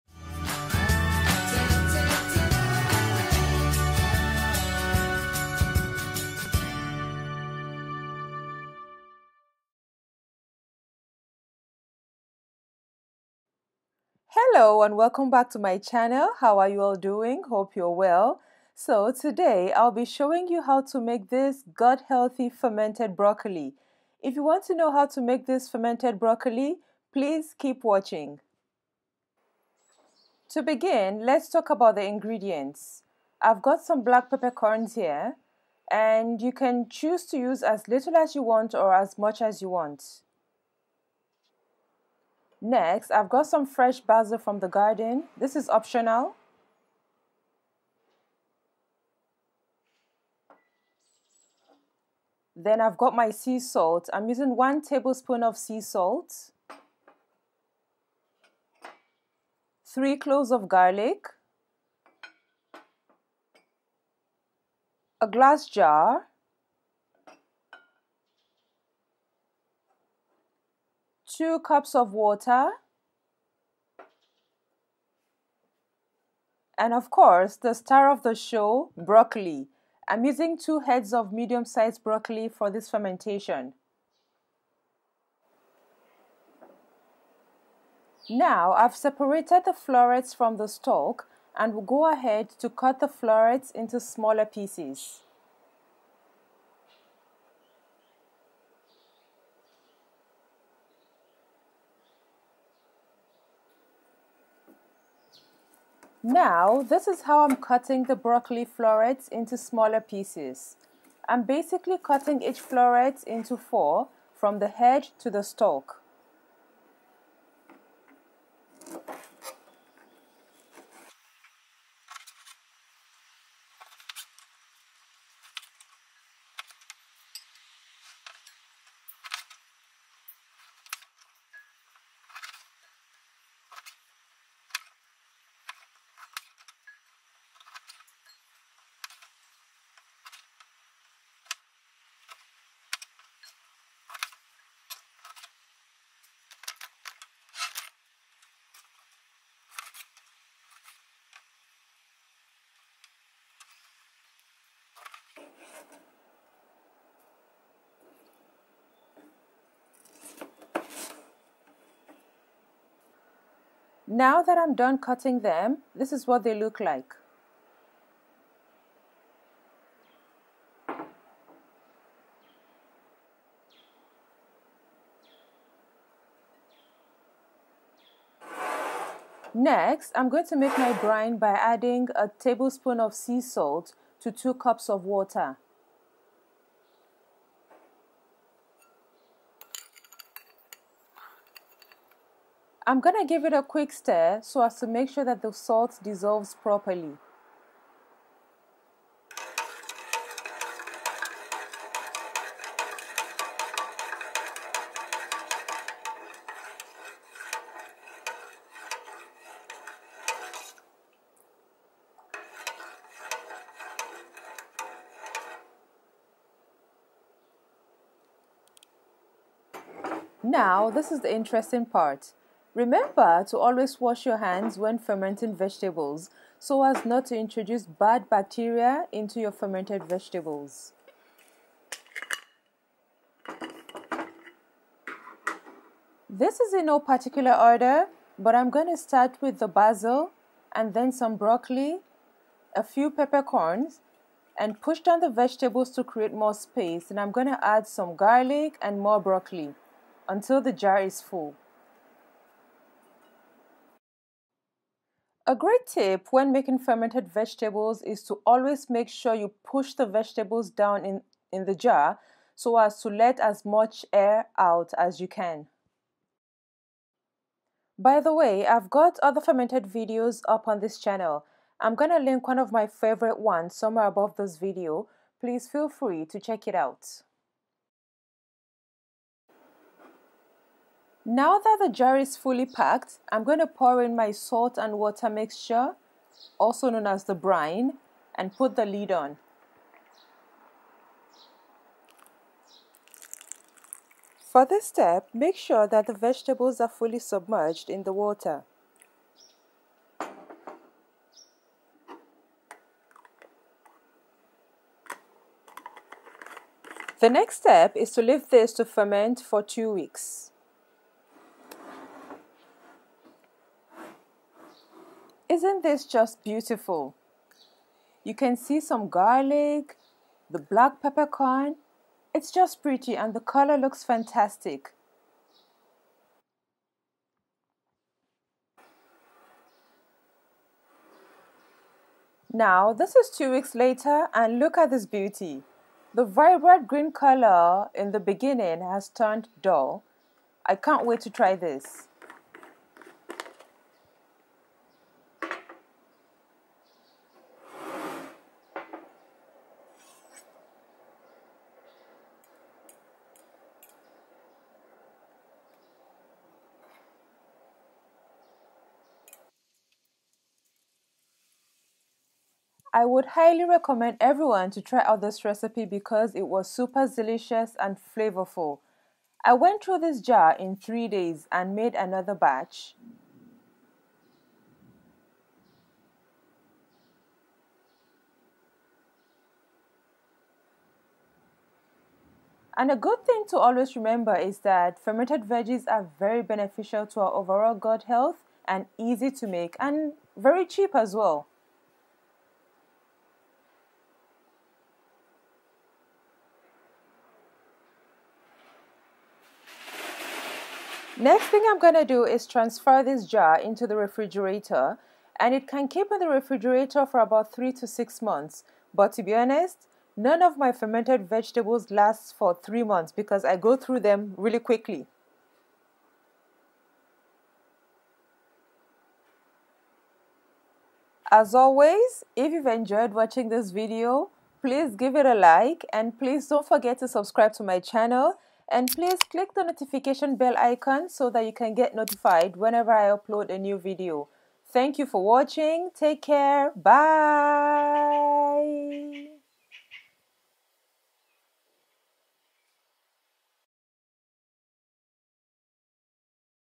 HOW TO FERMENT BROCCOLI | probiotic food for great gut health
This video is about how to ferment broccoli. Fermented broccoli is rich in probiotics and very good for our gut health and immunity. Here are a few benefits of fermented broccoli:
BENEFITS OF FERMENTED BROCCOLI
1. Rich in probiotics
2. Rich in b vitamins
3. Rich in vitamin c
4. Helps maintain an excellent gut health
5. Supports cellular health
6. Excellent antioxidant

INGREDIENTS
1. Broccoli
2. Black pepper corn
3. Garlic
4. Fresh basil
5. 1 tablespoon of salt
6. 2 cups of water

INSTRUCTIONS
1. Separate broccoli florets from stalk and cut to smaller bits
2. Rinse thoroughly with water and drain excess water using a strainer
3. Make brine by adding sea salt to 2 cups of water and stir to dissolve
4. Transfer broccoli, basil, pepper corns and garlic to jar , make sure the jar is tightly packed
5. Add brine to vegetables ensuring the vegetables are fully submerged in water
6. screw on the lid and leave on a counter to ferment for 2 weeks
7. After 2 weeks transfer to refrigerator and enjoy
*** once open transfer to refrigerator
***fermented broccoli can last in the refrigerator for up to 3-6 months
***Always wash your hands when fermenting vegetables
***place a try/plate under the jar during fermentation as the fermentation process could cause brine to bubble over